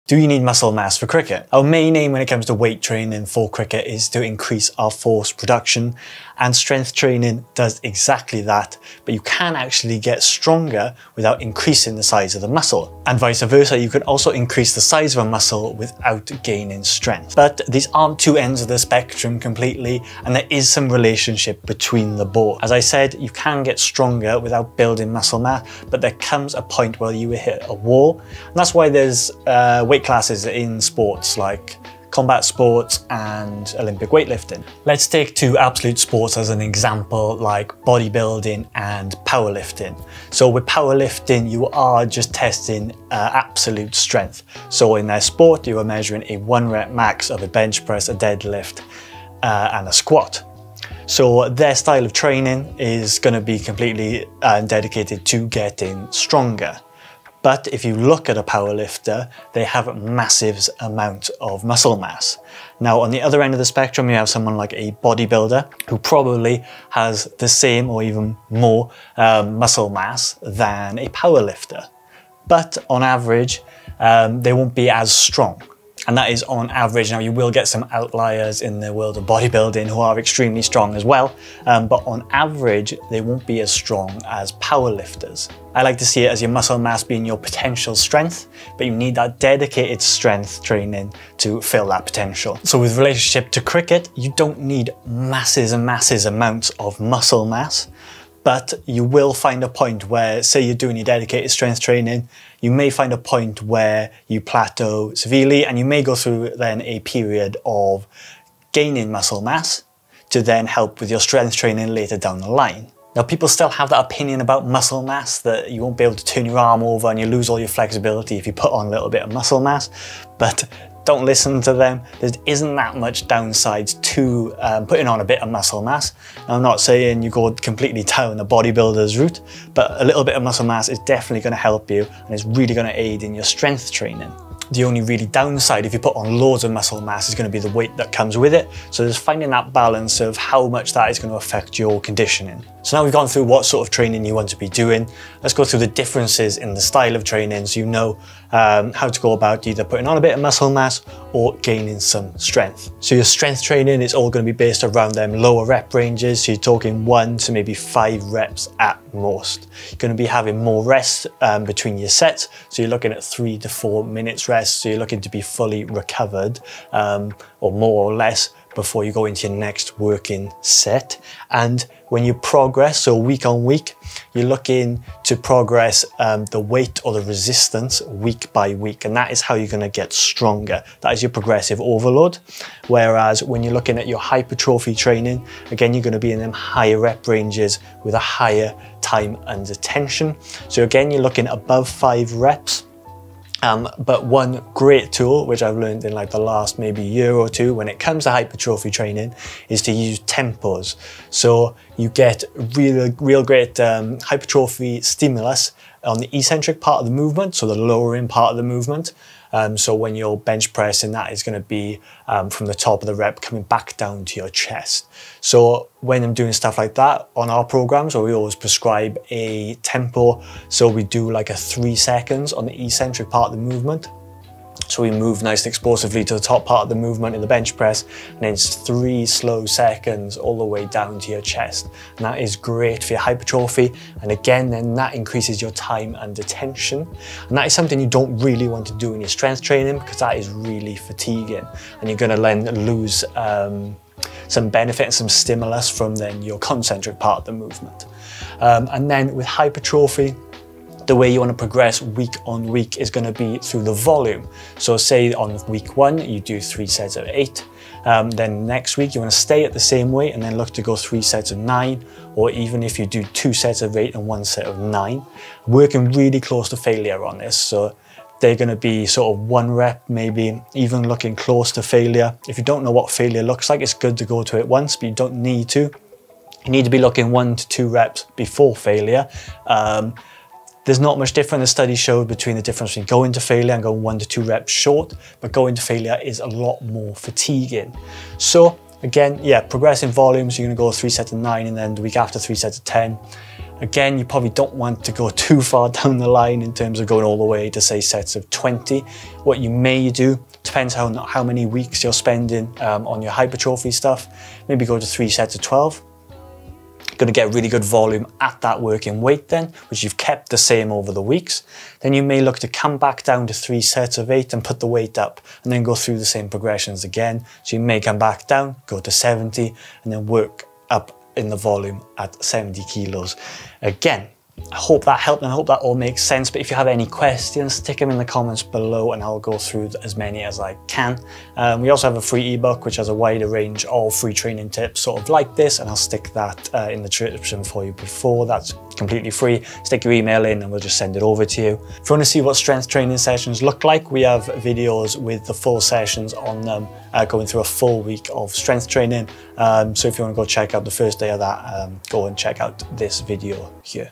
Do You Need Muscle Mass For Cricket?
A deep dive into the arguement of needing muscle mass for cricket.